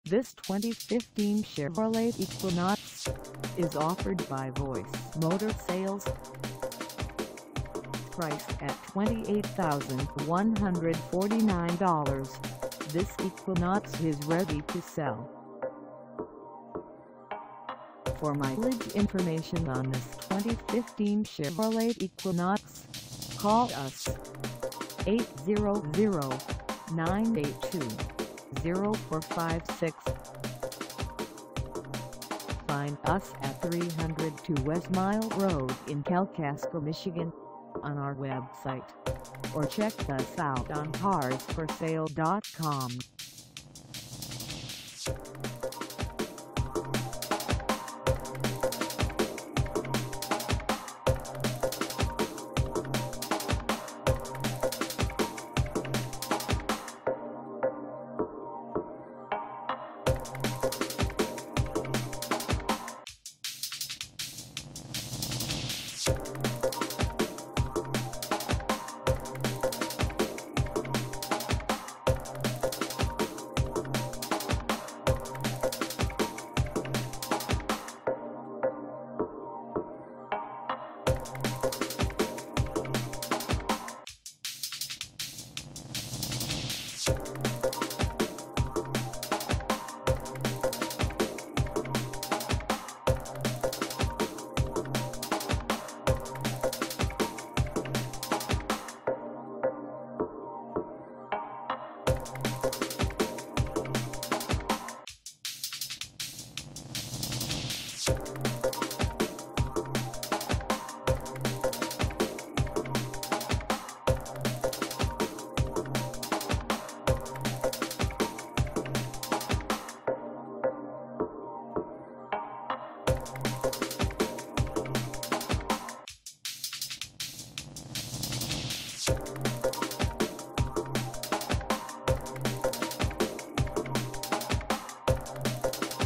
2015 Chevrolet Equinox LT AWD 4dr SUV w/1LT for sale in Kalk
This 2015 Chevrolet Equinox LT AWD 4dr SUV w/1LT is for sale in Kalkaska, MI 49646 at Voice Motor Sales.
Engine: I4 2.4L I4
Transmission: Automatic 6-Speed
Color: Summit White
Mileage: 0
Contact Voice Motor Sales 302 W Mile Rd Kalkaska, MI 49646 to test drive this 2015 Chevrolet Equinox LT AWD 4dr SUV w/1LT today.